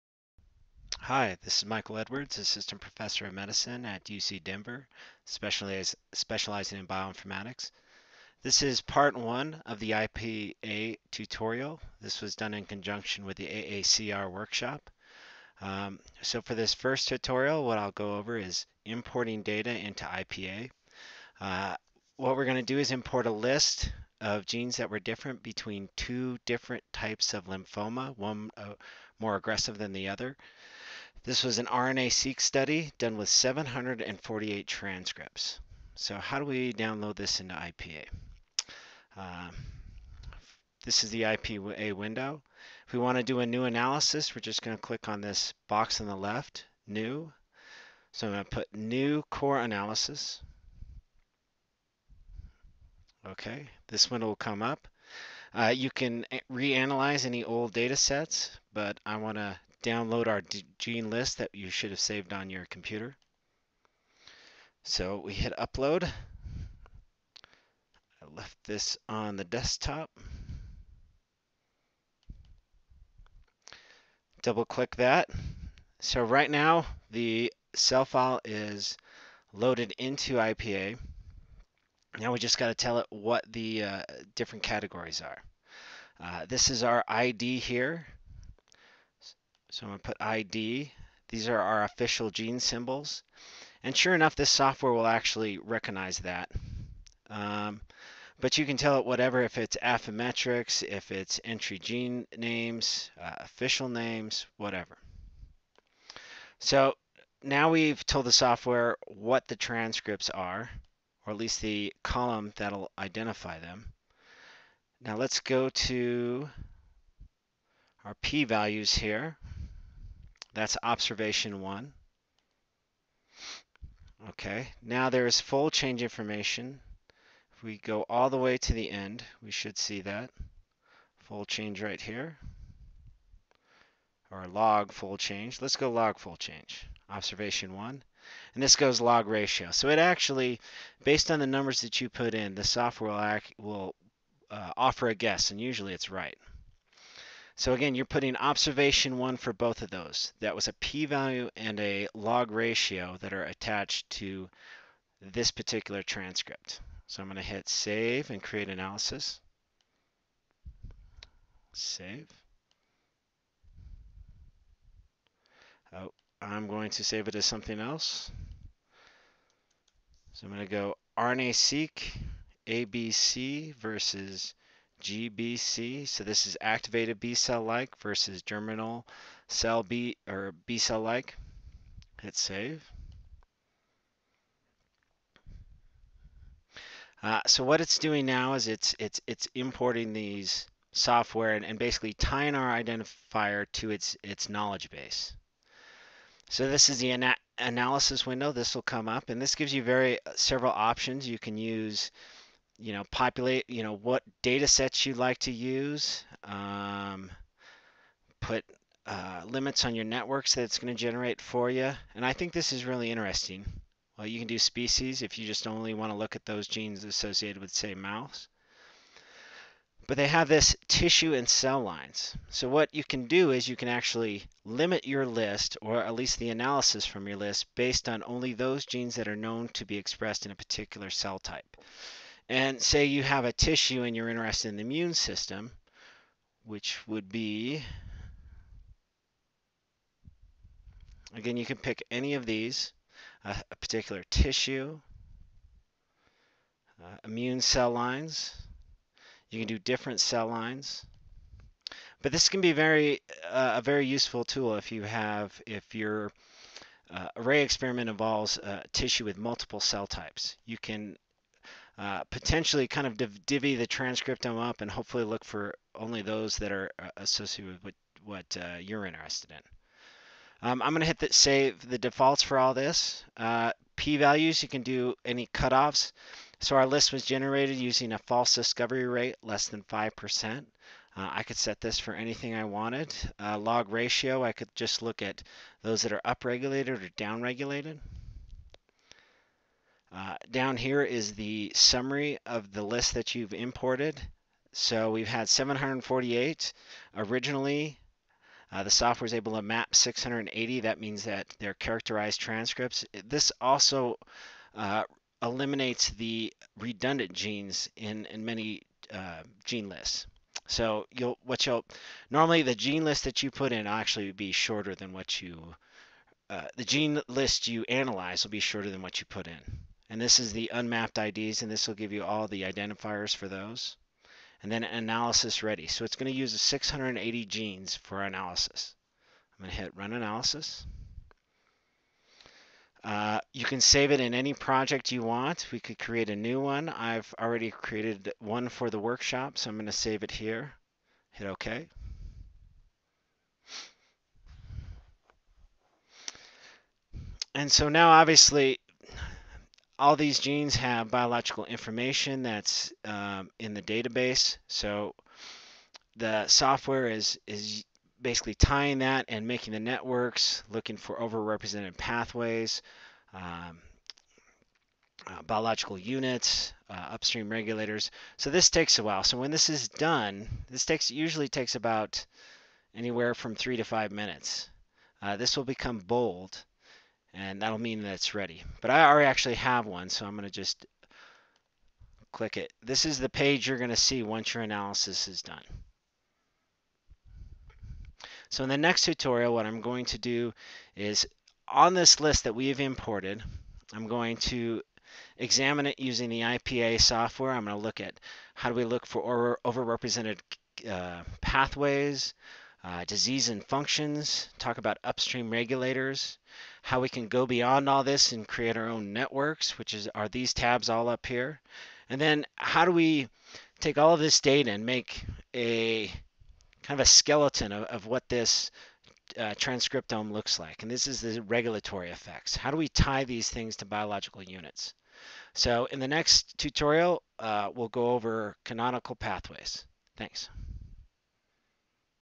AACR Workshop 2014_IPA-1_UploadingData
IPA_Part1: A series of video tutorials that complement the "Using Genomics and Bioinformatics in Cancer Research" laboratory for the 23rd Annual AACR Workshop: Molecular Biology in Clinical Oncology.  This tutorial covers importing a gene list into the bioinformatics platform Ingenuity Pathway Analysis (IPA)